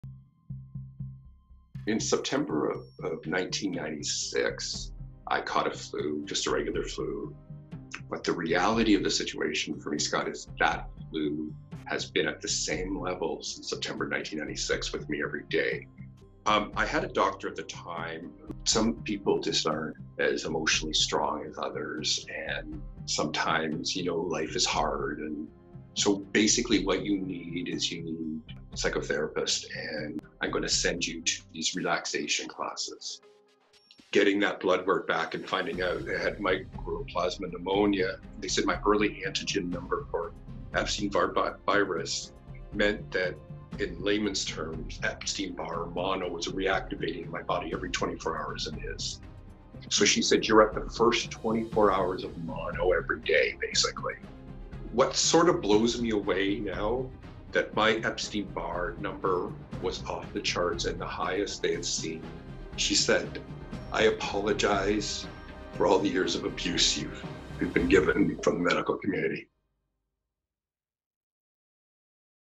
Jeff Smith trailer: COVID Gaslighting Warning: If you don’t recover, doctors may call psychosomatic
Imagine back to when you had a bad flu. Remember the nausea, head spins, pain and soreness, vomiting and diarrhea, and crushing fatigue? Remember how the worst lasted for a few days? Now imagine having that flu for a few weeks. Or a few months. Or like Jeffery Smith, for a few decades.
Now think of the millions of COVID patients. A good number of the survivors are not fully recovering from this ‘flu’ bug. They remain sick for weeks. Soon they will have been sick for months. And if they are unlucky like Jeffery, they may be permanently sick and disabled, and the health care system will effectively abandon them.
Most doctors didn’t take Jeff’s ongoing flu symptoms seriously, dismissing them and subtextually blaming Jeff for being emotionally weak, or not trying hard enough, or not really wanting to be healthy. You know, gaslighting.
So when Jeff started to have bowel symptoms, he didn’t tell his doctor. Who needs to be invalidated and gaslighted? But it turned out Jeff had a tumour and needed surgery and suddenly the health care he received was empathic, prompt and validating. A stark contrast to the years of dismissal and gaslighting of his flu symptoms.
As Jeff explains, the tumour for which he received exemplary care only impacted his quality of life a fraction compared to how the never ending flu shattered his future. And as the COVID deaths and infections continue to rise, the people who do not recover may also experience disbelieving doctors, and a health care system that doesn’t care.